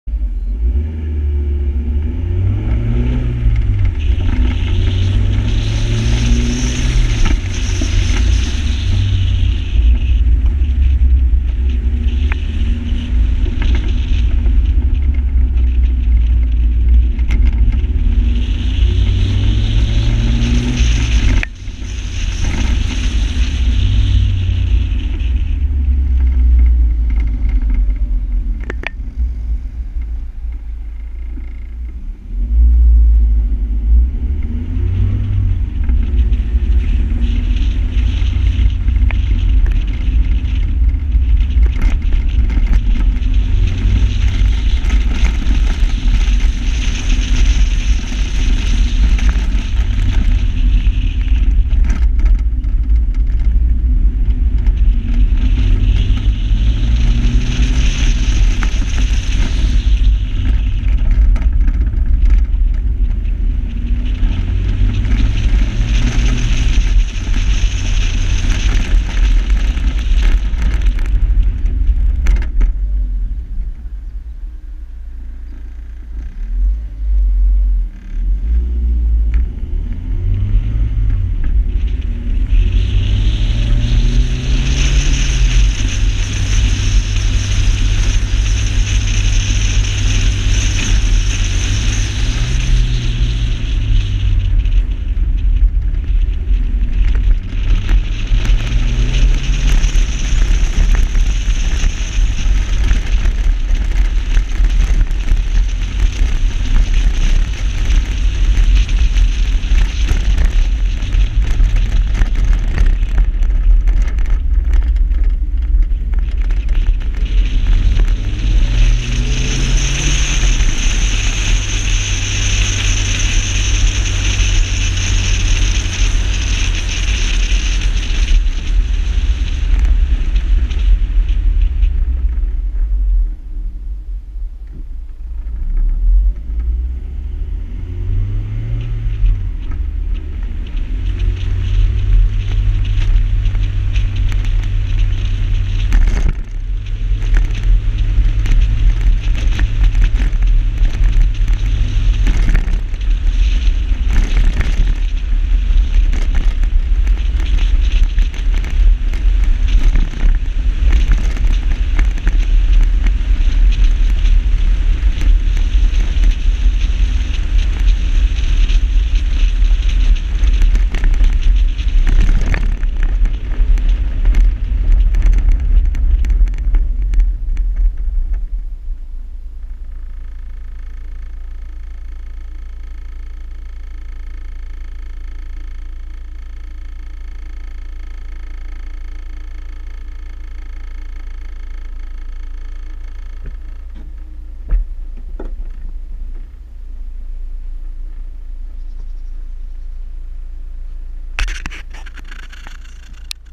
Waimate 50 Sprint Track 2014
A lap of the new waimate 50 sprint track for 2014